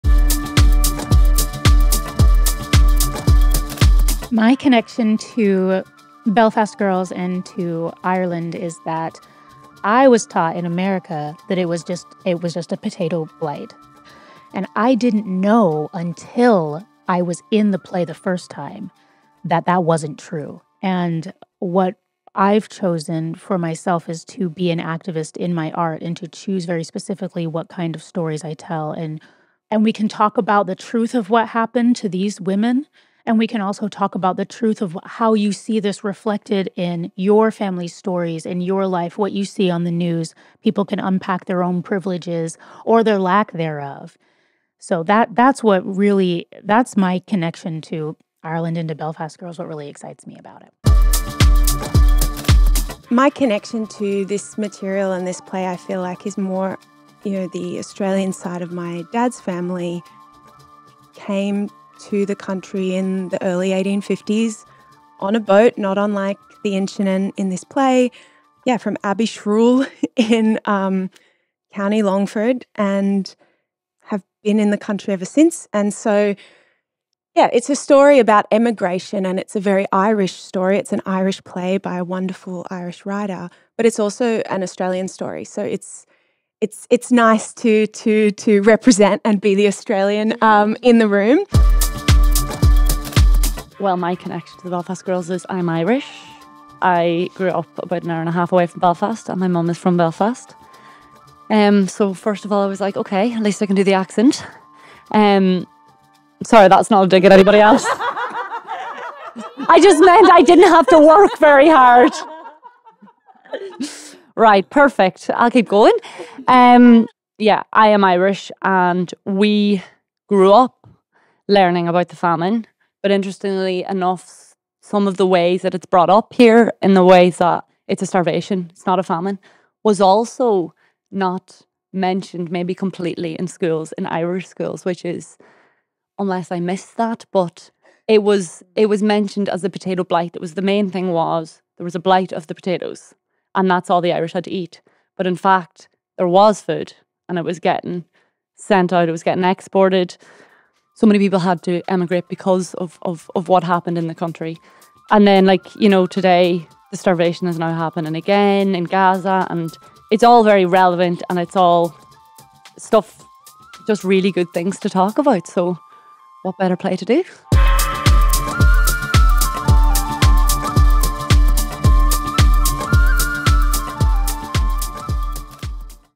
BELFAST GIRLS by Jaki McCarrick: Cast Interview - What connections do you have to the play?
In this interview our Cast of BELFAST GIRLS by Jaki McCarrick, talk to us about what their own personal connections are with play, including their characters and where the play is set; Ireland. Some fascinating discoveries!

Part of Zeitgeist Irland 24, an initiative of Culture Ireland and the Embassy of Ireland in Germany. Partially funded by the Landeshauptstadt Düsseldorf and Sparkasse Düsseldorf Kunst- und Kulturstiftung.